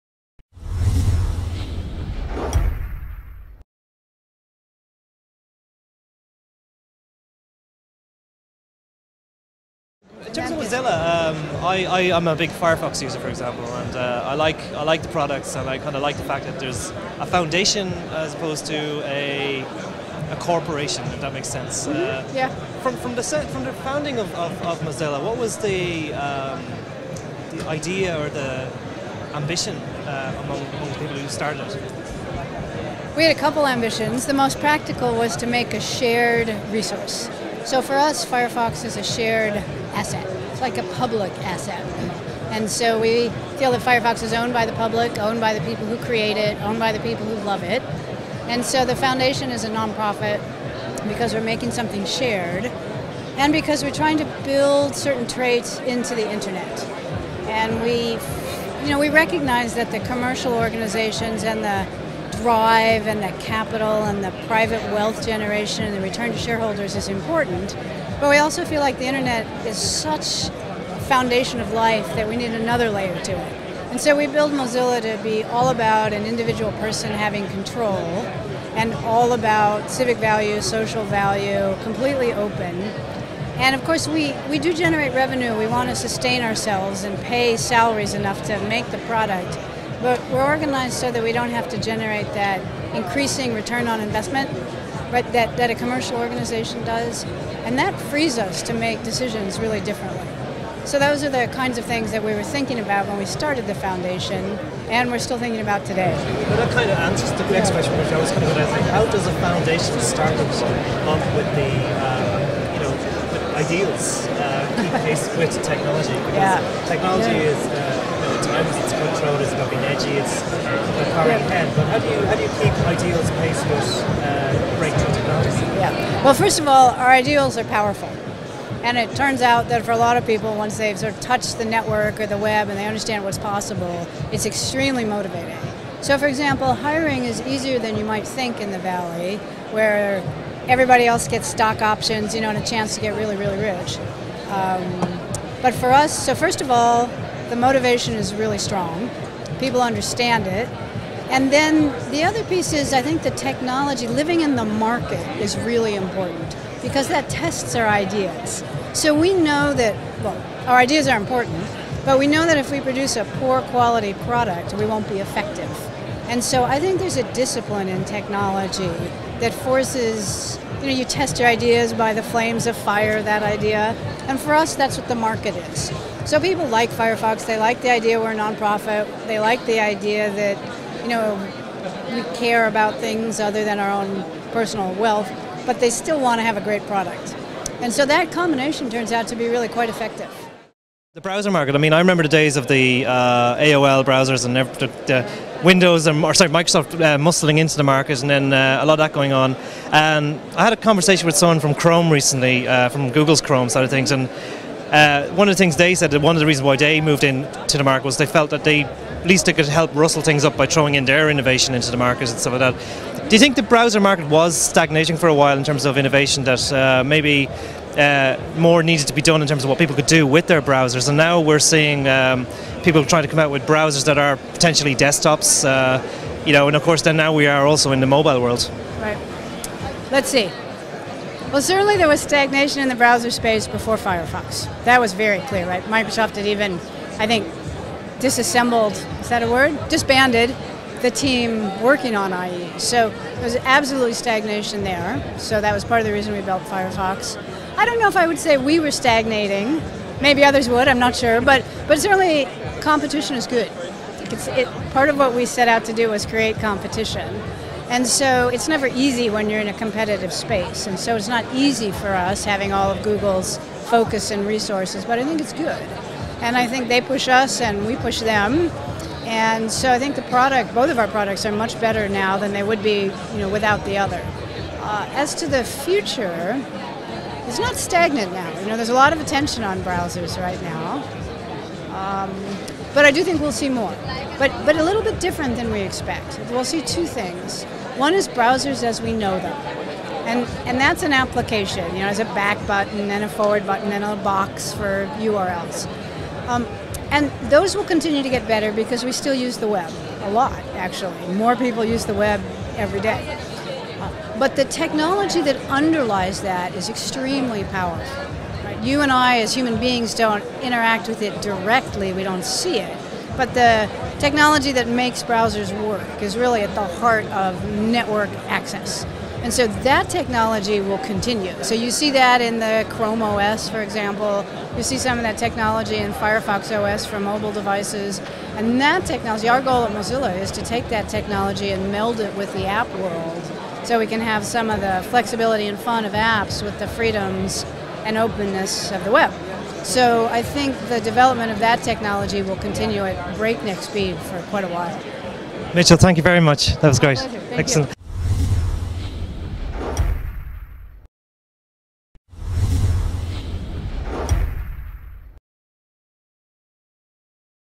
Dublin Web Summit | Mitchell Baker
John Kennedy talks to Mozilla founder Mitchell Baker at the Dublin Web Summit